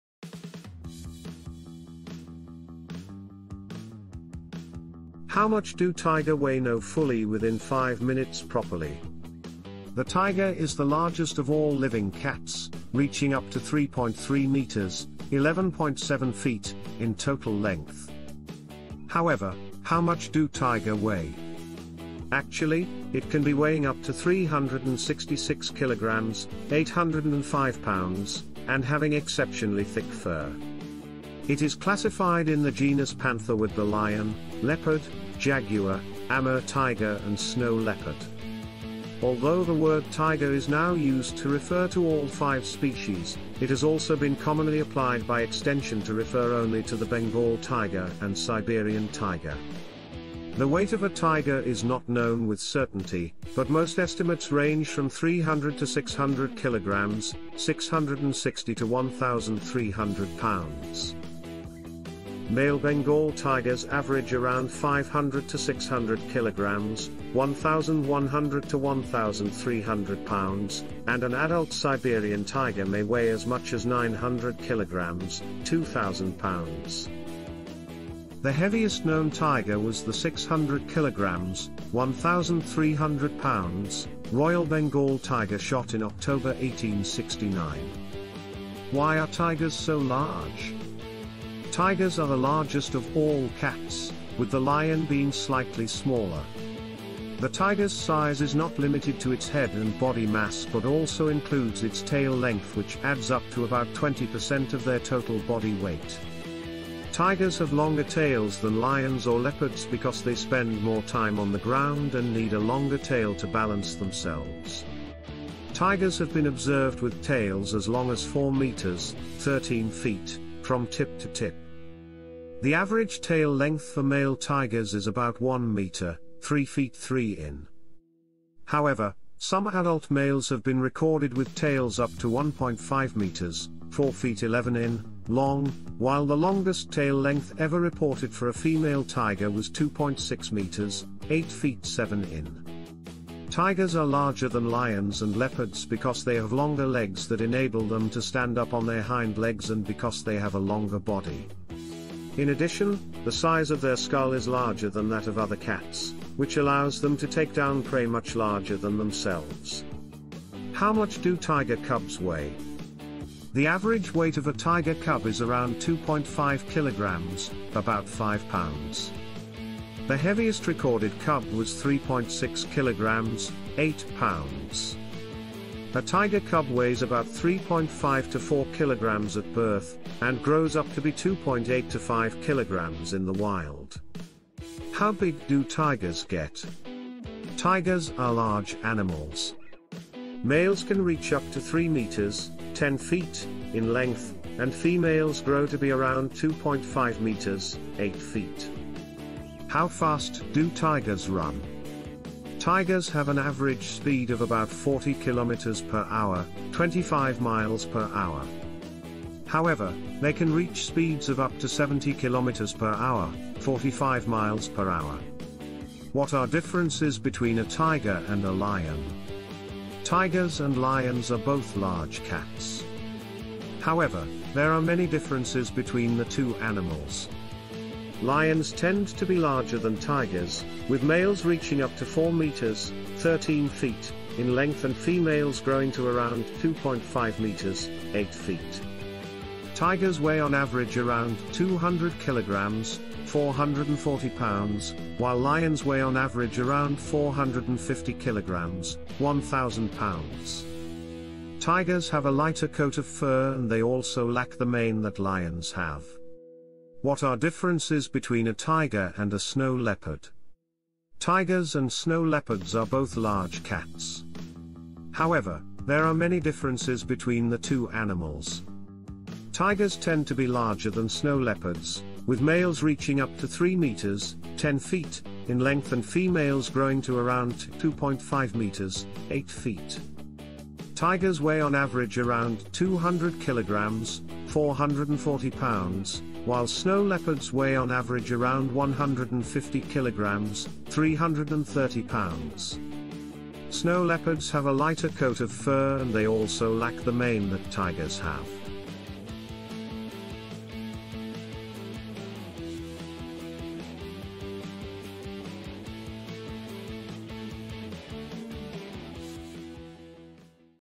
How Much Do Tiger Weigh-Know Fully Within 5 minutes Properly
The tiger is the largest of all living cats, reaching up to 3.3 metres (11.7 ft) in total length. However, how much do Tiger weigh? Actually, it can be weighing up to 366 kg (805 lb), and having exceptionally thick fur. It is classified in the genus Panther with the lion, leopard, jaguar, Amur tiger and snow leopard. Although the word "tiger" is now used to refer to all five species, it has also been commonly applied by extension to refer only to the Bengal tiger and Siberian tiger.